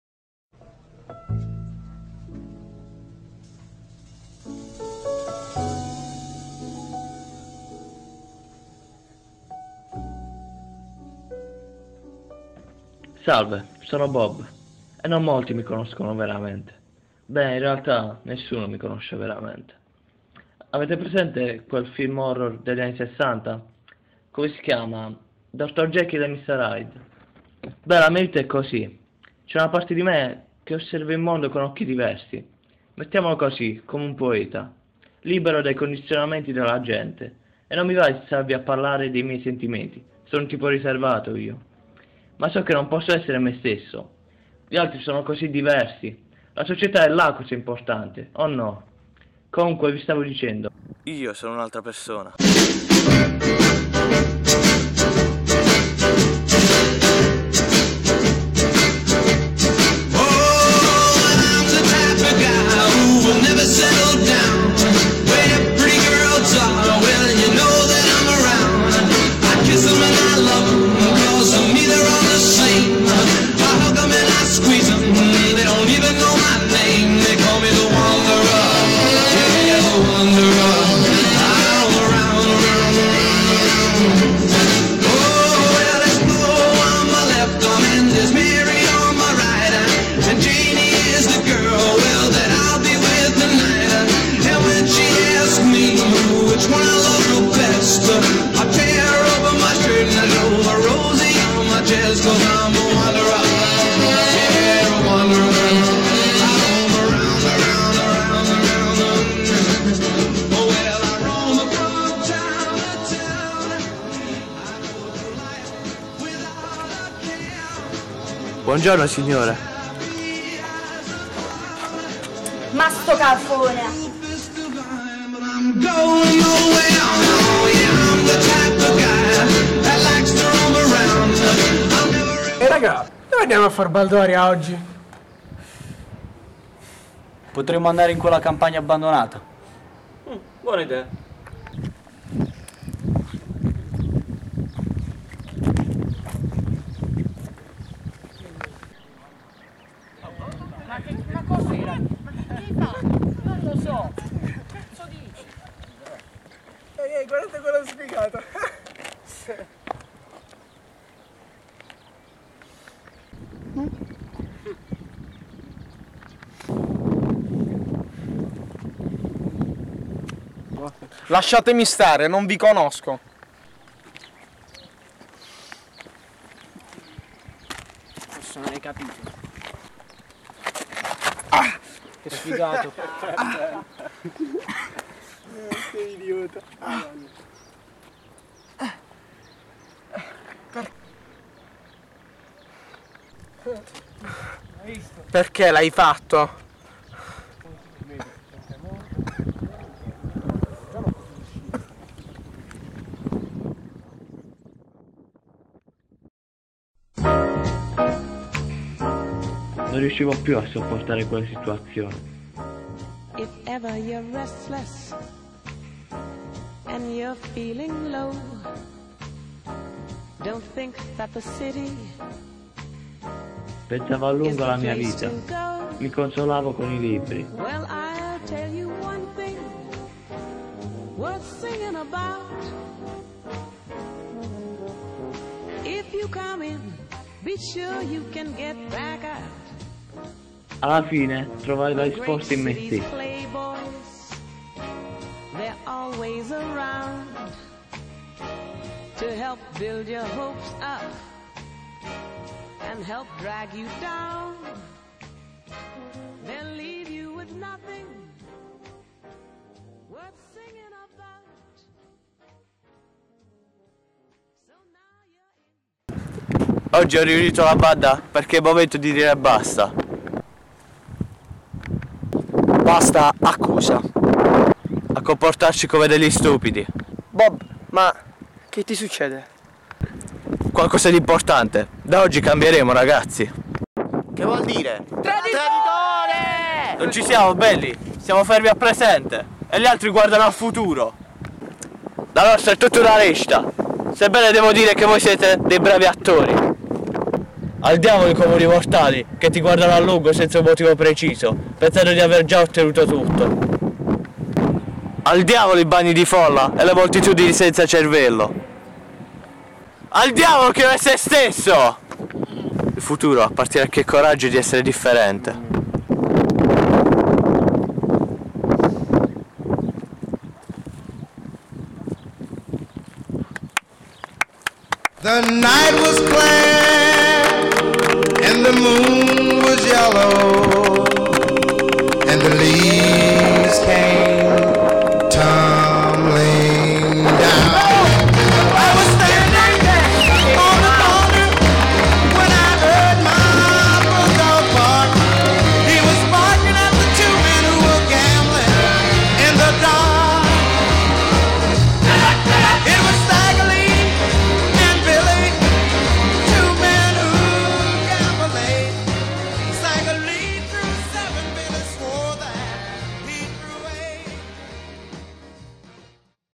MR DOUBLE CIA Company
Bob affronta la sua doppia personalità in un percorso illuminante... Ultimo lavoro di una infaticabile CIA che sa, sebbene gli errori, sempre migliorarsi.
GRAZIE DI CUORE A TUTTI COLORO CHE CI CREDONO E CONTINUERANNO A FARLO.